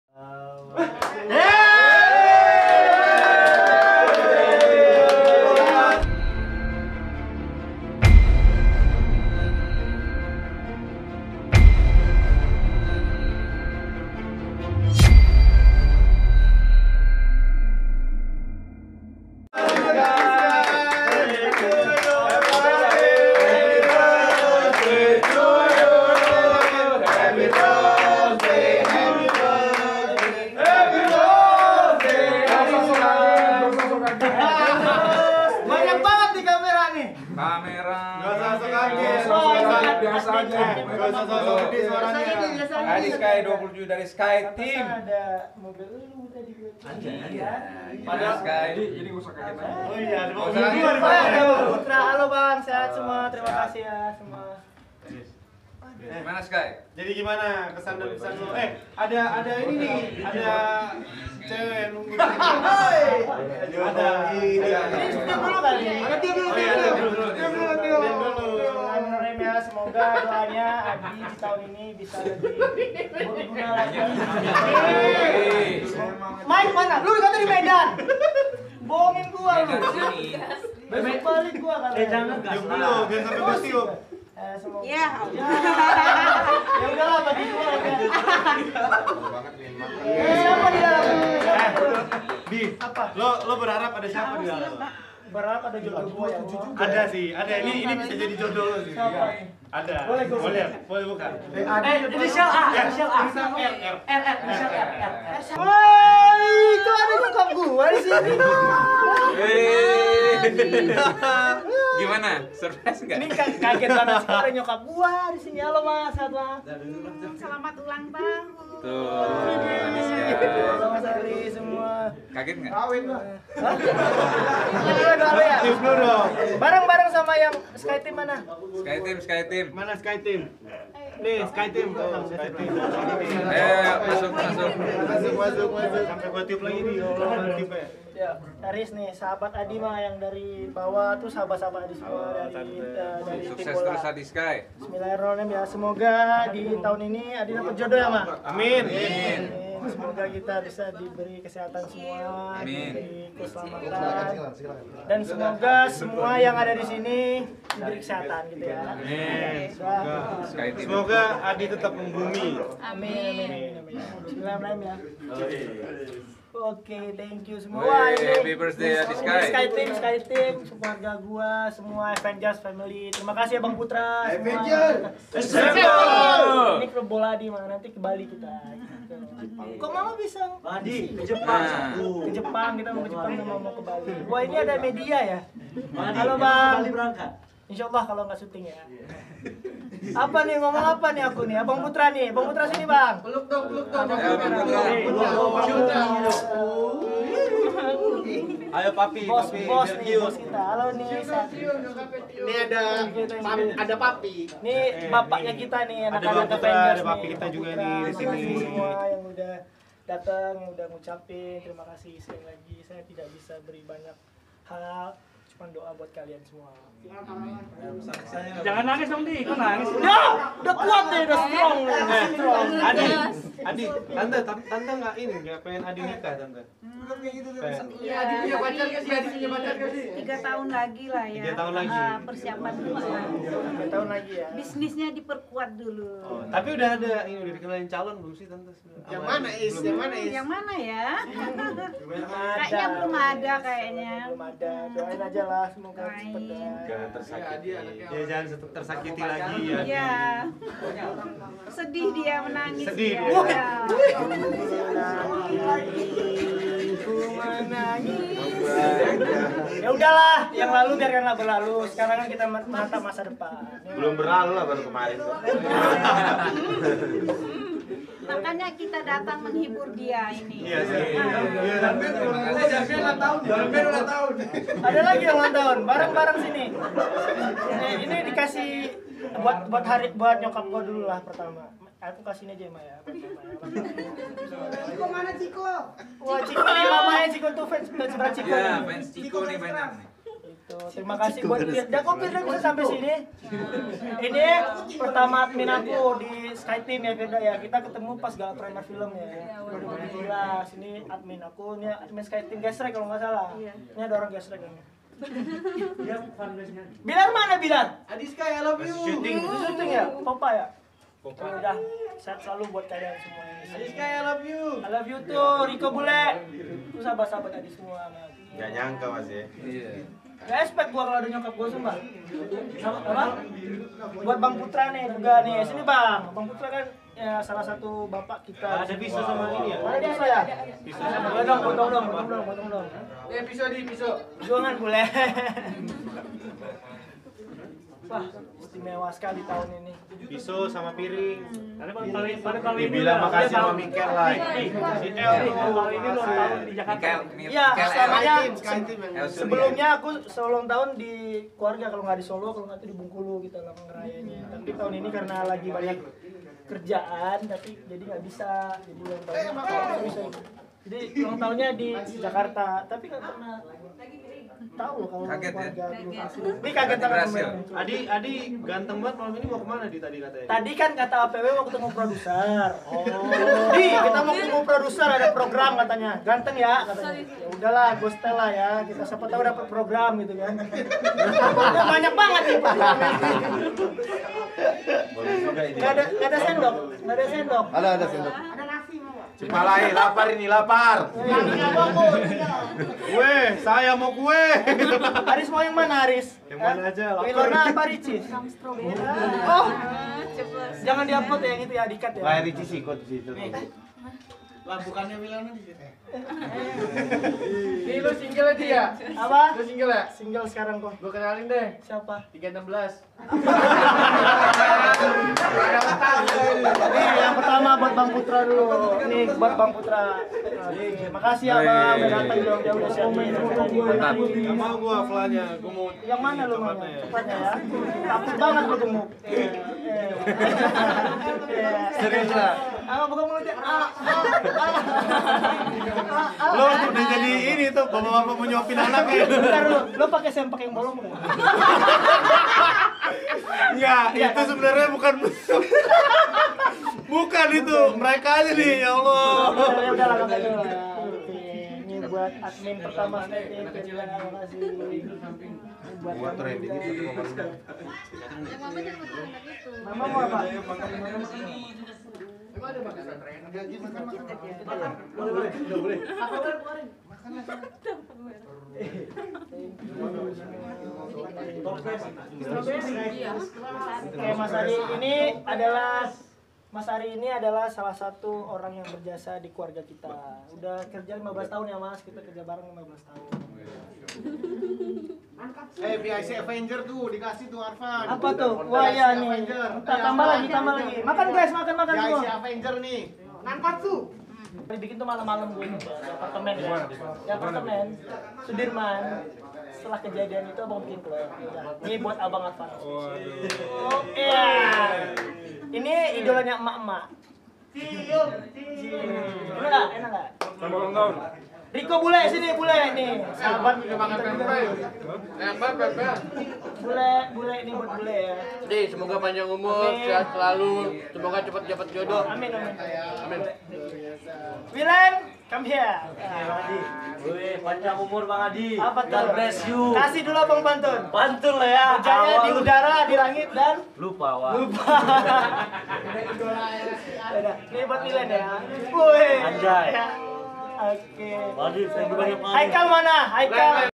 ADY SKY MENDAPAT KEJUTAN ULANG TAHUN DARI MAMA DAN HARRIS VRIZA SERTA AVENGERS
Ady Sky mendapat kejutan ulang tahun dari sang mama dan sahabat seperti Harris Vriza, Ardan Afif dan lainnya.